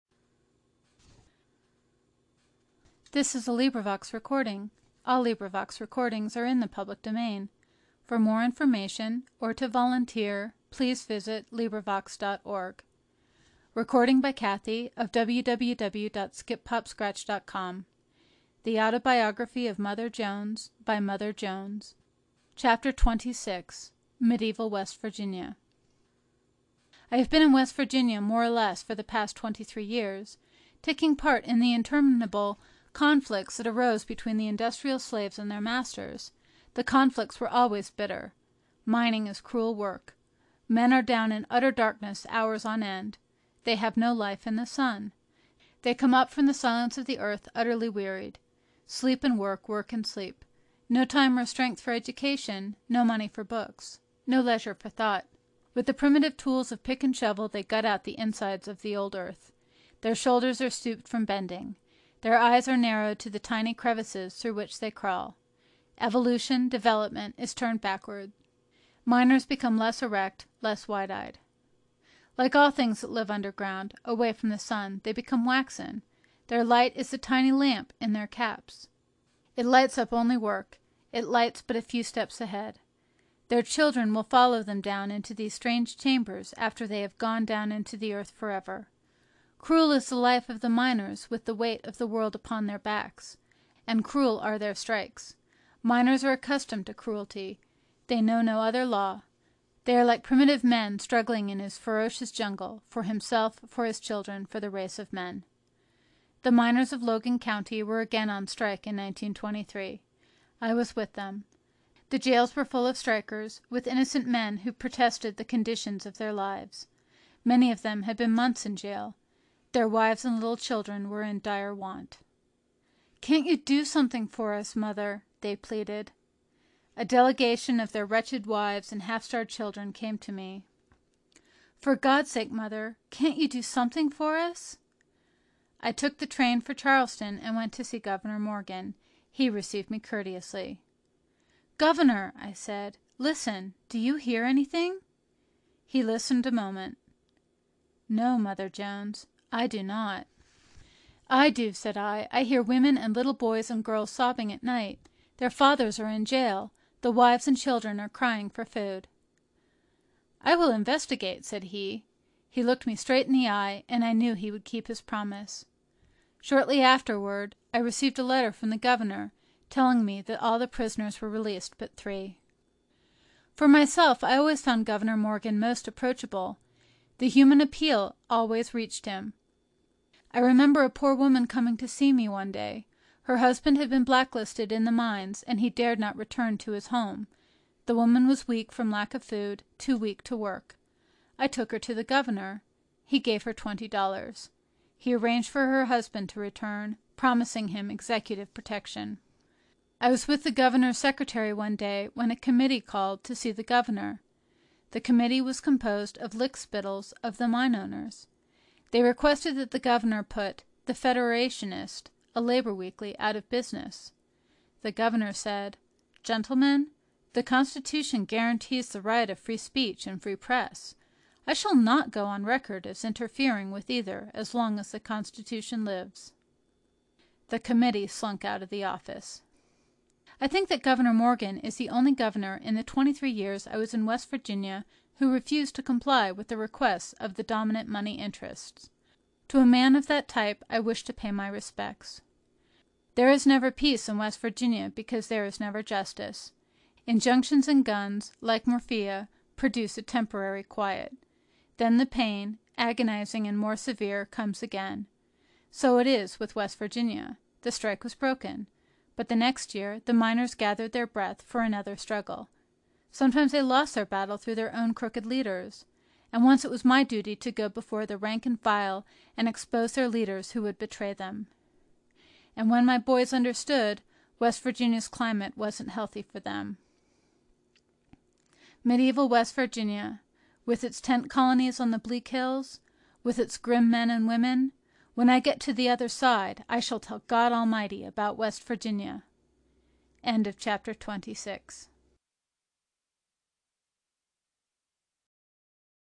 The Autobiography of Mother Jones by Mary Harris Jones 📚 |Audiobook-Chapter 26 | Powerful Audiobooks
Explore the life and legacy of Mary Harris Jones, better known as Mother Jones, a legendary labor organizer and activist. A founding member of the International Workers of the World (IWW) and a key figure in the United Mine Workers and the Socialist Party of America, Mother Jones dedicated her life to fighting for workers' rights. 🌟

The Autobiography of Mother Jones
📚 Author: Mary Harris Jones
🌟 Genre: Autobiography, Biography, History
🌐 Language: English

Key Highlights:

Chronicles the life and activism of Mother Jones.
Explores her role in founding the IWW and her involvement with the United Mine Workers and the Socialist Party of America.
Provides insights into the labor movement and social justice struggles of her time.

Immerse yourself in the powerful narrative of Mother Jones and understand her impact on labor rights and social justice. 🌐